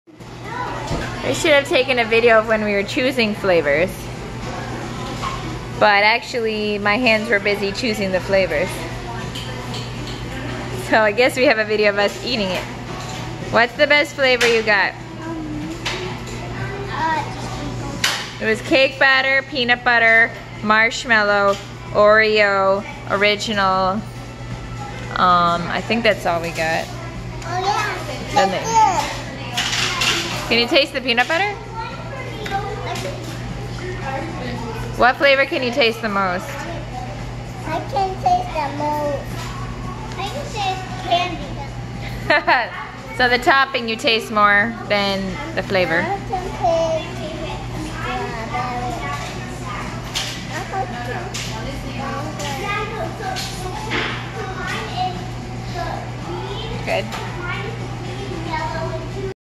16 Handles, can kids distinguish the flavors?
snack vlog